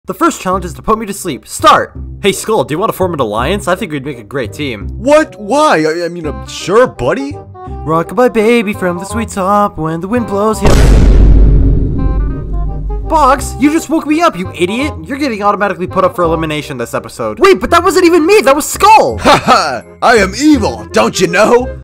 Skull is EVIL! 💀
It's time for the first challenge in The Hourly Object Show! Box attempts to wake up the host, Clapboard, but his performance was ruined by Skull of all people! Truly evil!

Inspired by Battle For Dream Island by Jacknjellify, and The Daily Object Show by FusionAnimations